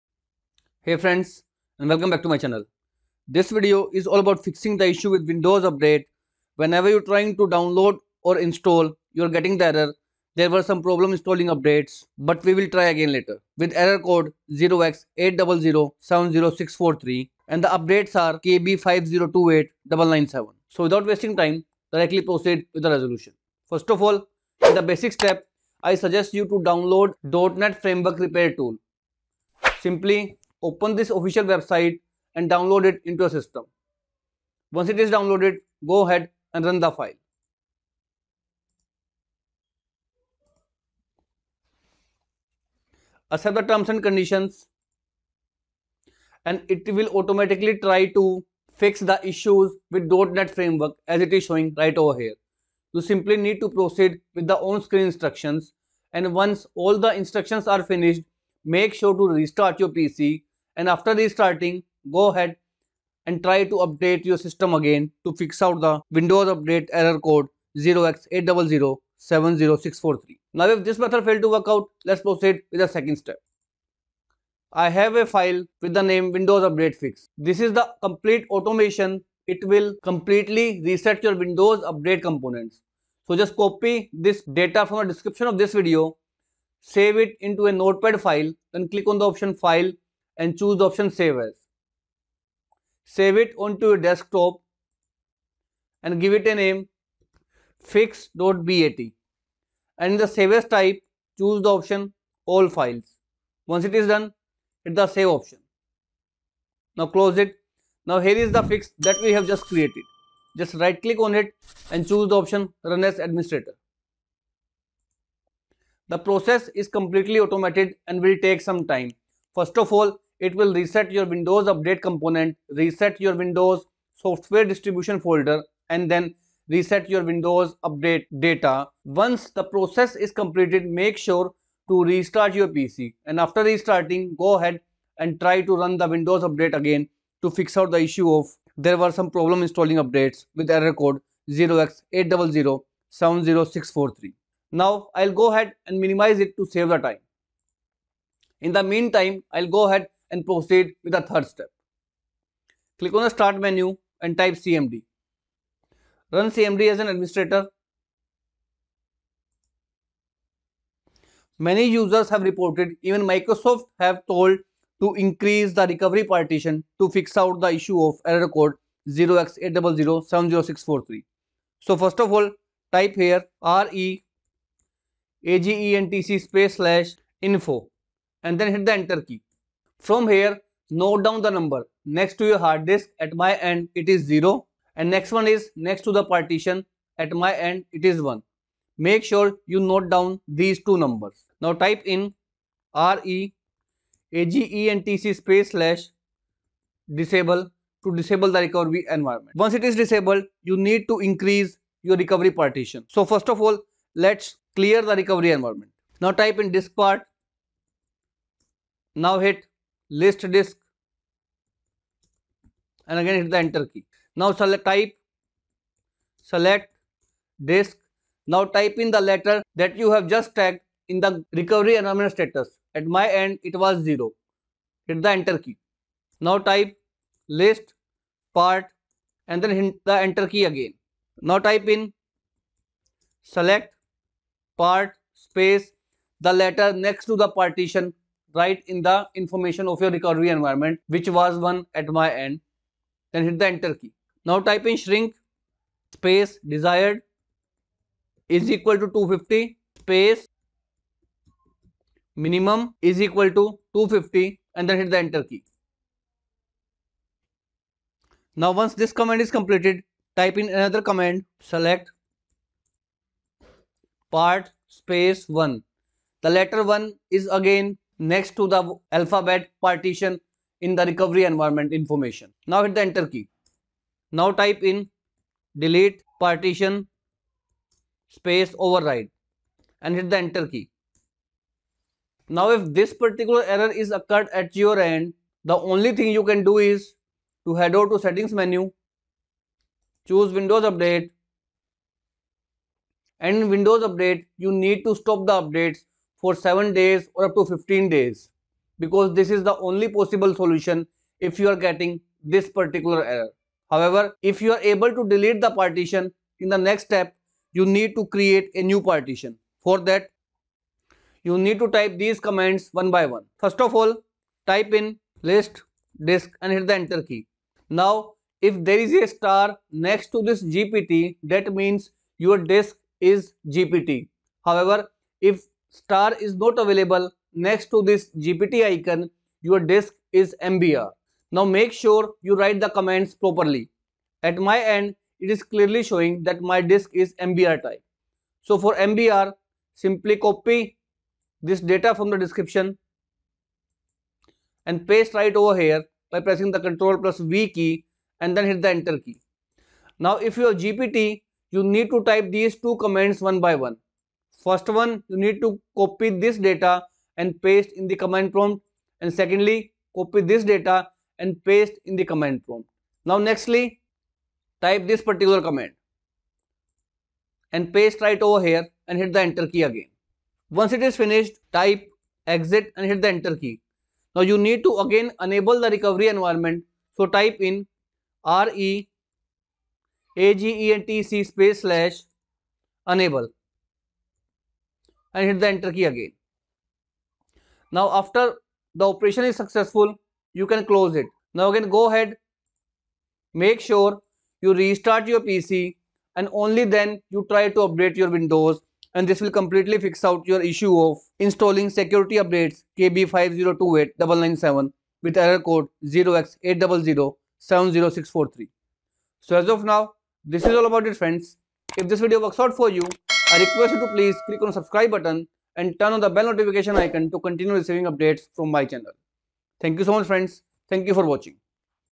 Windows update error code 0x80070643 (Security update KB5034441) Fix
While updating windows 10 users are getting error encountered with error code 0x80070643. The error message says "There were some problems installing updates but we will try again later. Error code 0x80080643" There were are security updates kb5028997 whose offline version is also not available.
The said error code is bug from the Microsoft which will be resolved soon but in the mean time you can try the below mentioned steps to fix windows update error 0x80070643.

00:00 Introduction
00:21 Method 1 by repairing the .net framework
01:10 Method 2 by resetting windows update components
02:30 Method 3 by increasing the recovery partition

commands to paste in command prompt:
reagentc /info
reagentc /disable

diskpart
list part
select disk X
list part X
select part X
shrink desired=250 minimum=250
select part X
delete partition override

To create/increase partition:
list disk
for GPT Type in:
create partition primary id=de94bba4-06d1-4d40-a16a-bfd50179d6ac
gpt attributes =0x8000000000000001

For MBR type in:
create partition primary id=27

format quick fs=ntfs label=”Windows RE tools”
exit
reagentc /enable